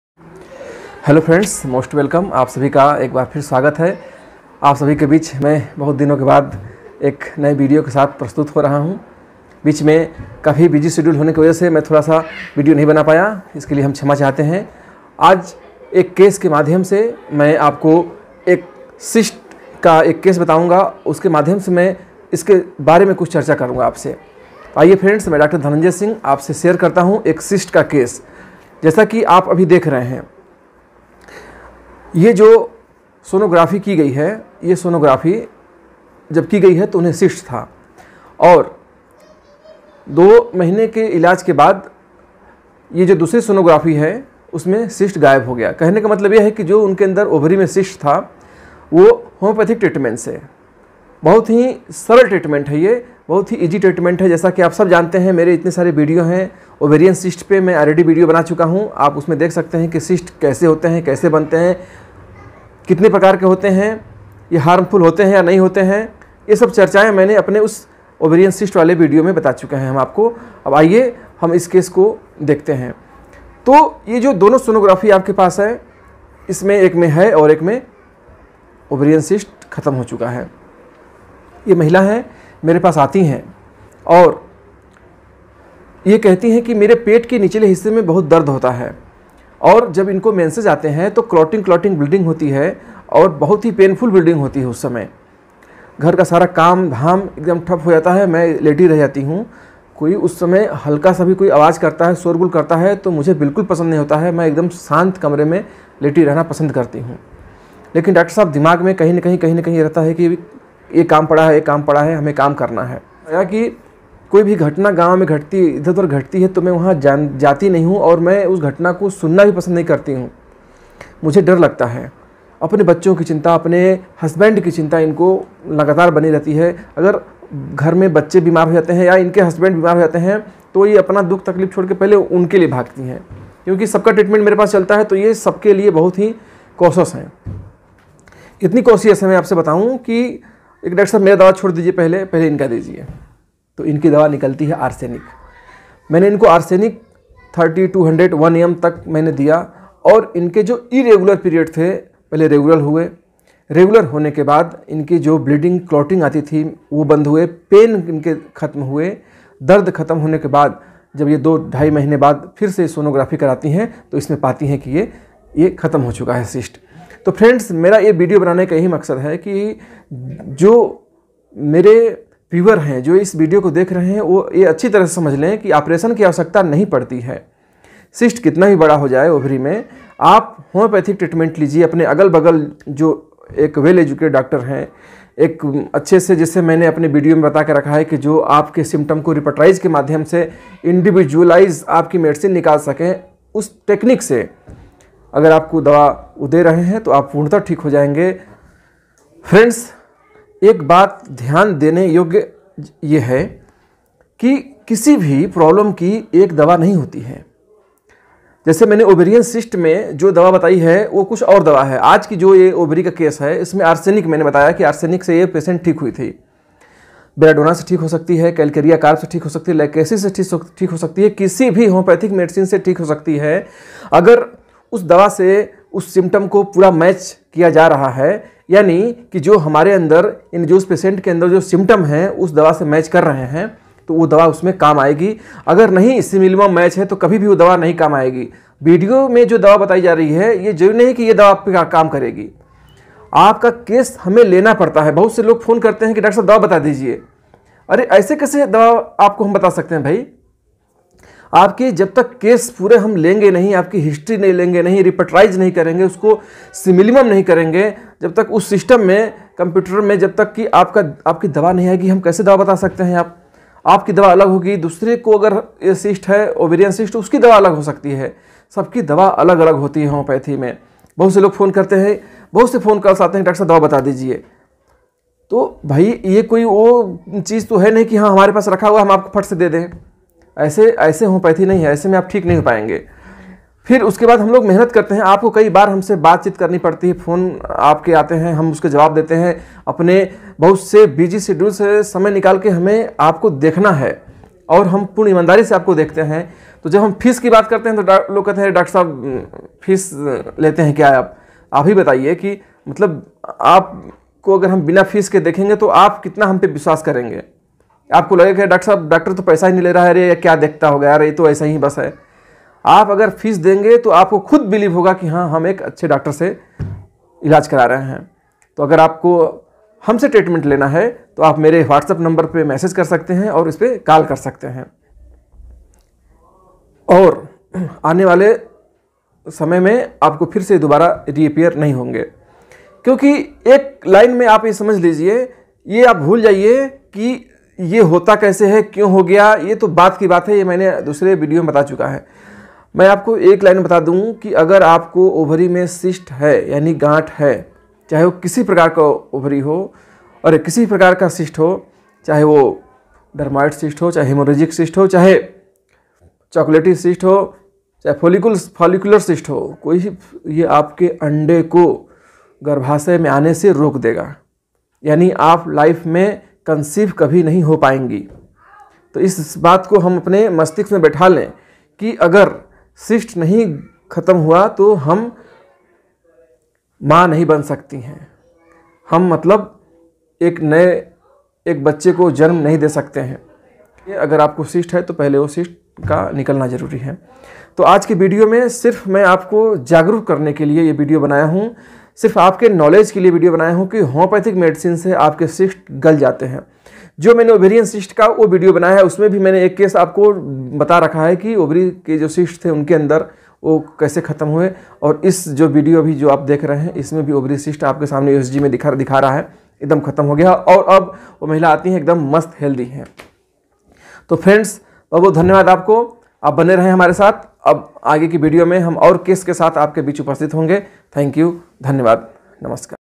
Ovarian Cyst completely cured case | ओवरी में गांठ का सफल इलाज
This video is about ovarian cyst, which is very common in females now a days. homeopathic treatment of ovarian cyst more success through computerized repertisation system.
Skin Diseases Homeopathic Treatment: Ovarian Cyst completely cured case | Ovarian Cyst | Ovary me ganth